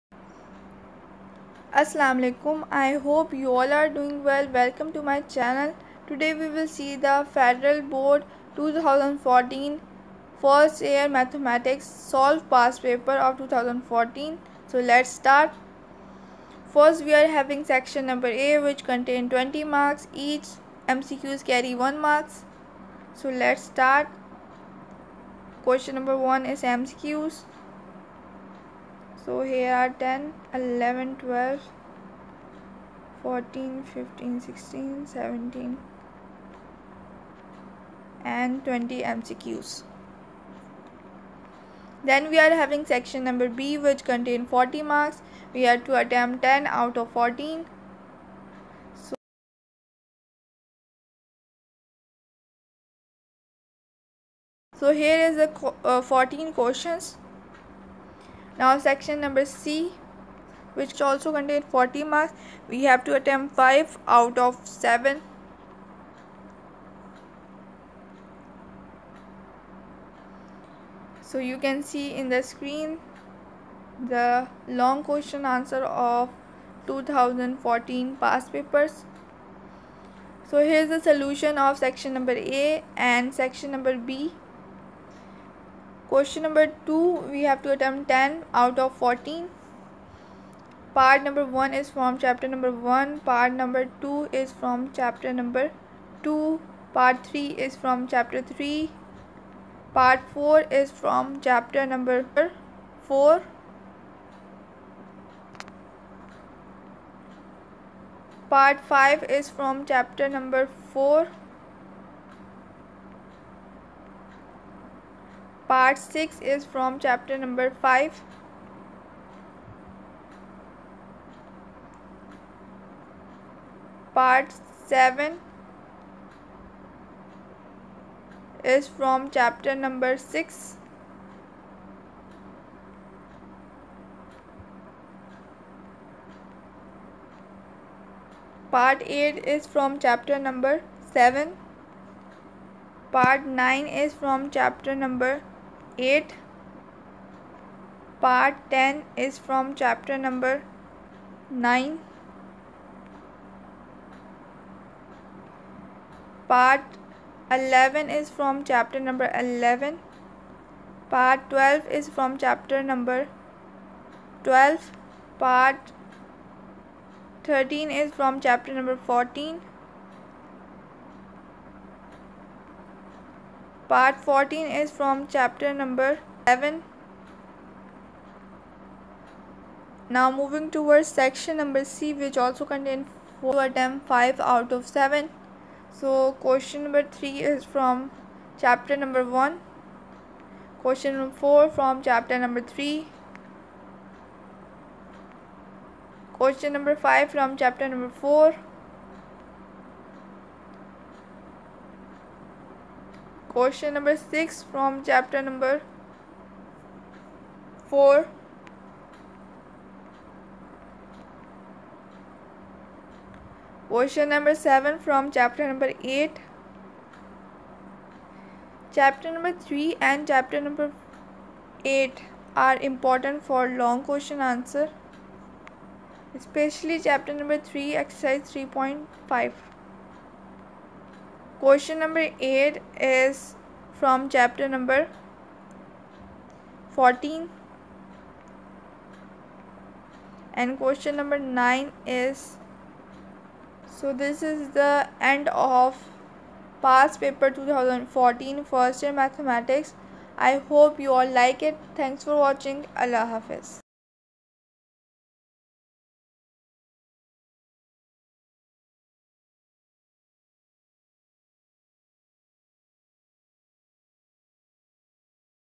Fsc- 1 Math | Solved Past Paper |2014 | Class 11th Math | First Year | Reduced Syllabus
Aoa
Everyone Hope you all are fine and Focusing on Your studies and having a good & quality time with your family...

This lecture is based upon the
First  year ( Fsc part 1) Federal board
 2020- 21 Reduced syllabus.This lecture is based upon the
First  year ( FBISE part 1) Punjab text book board/KPK text book board/ Sindh text book board inter part 1 physics book 1 lecture is conducted in Urdu/hindi/English.
2020- 21 Reduced syllabus.

Mathematics SOLVED PAST PAPER  2014

newbook20-21 11th Math